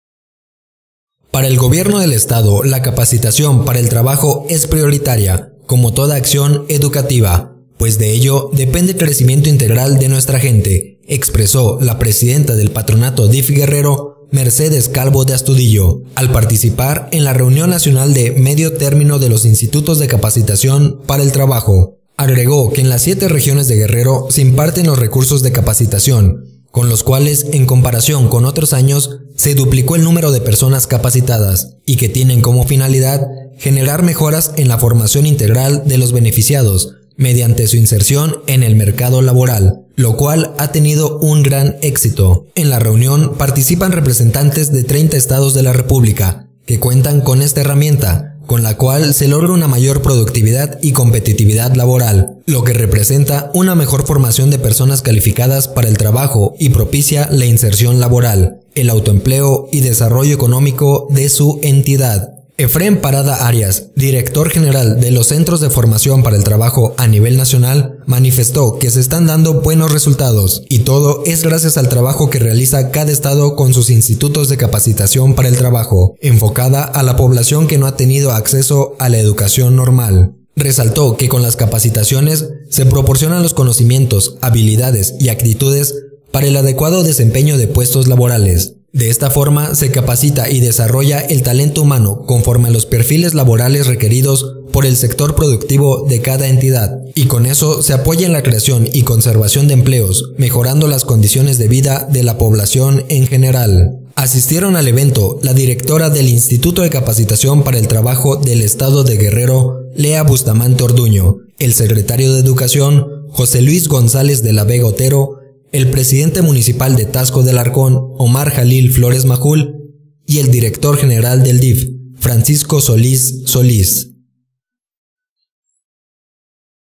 La capacitación para el trabajo prioridad en el gobierno del estado Mercedes Calvo
Para el gobierno del estado la capacitación para el trabajo es prioritaria como toda acción educativa, pues de ello depende el crecimiento integral de nuestra gente, expresó la presidenta del Patronato DIF Guerrero, Mercedes Calvo de Astudillo, al participar en la Reunión Nacional de Medio Término de los Institutos de Capacitación para el Trabajo.

 Resaltó que con las capacitaciones se proporcionan los conocimientos, habilidades y actitudes para el adecuado desempeño de puestos laborales, de esta forma se capacita y desarrolla el talento humano conforme a los perfiles laborales requeridos por el sector productivo de cada entidad, y con eso se apoya en la creación y conservación de empleos, mejorando las condiciones de vida de la población en general.